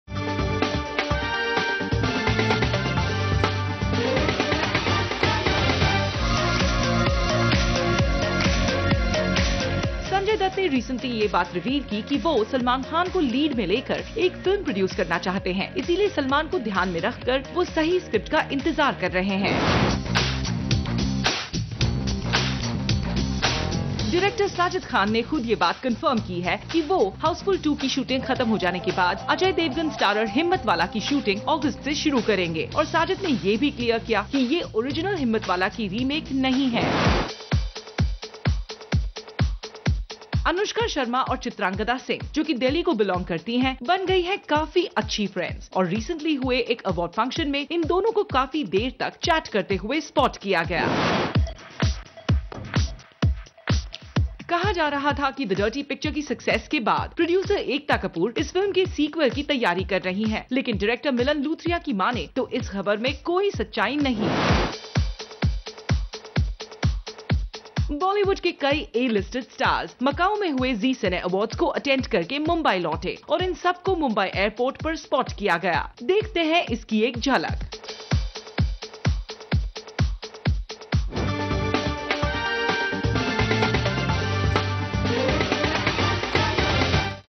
Salman Khan to Star in Sanjay Dutt's Film
My Upcoming film is not A Remake of Himmatwala says Sajid Khan
Anushka & Chitrangada at Award Night
Bollywood Stars Return from Zee Cine Awards 2012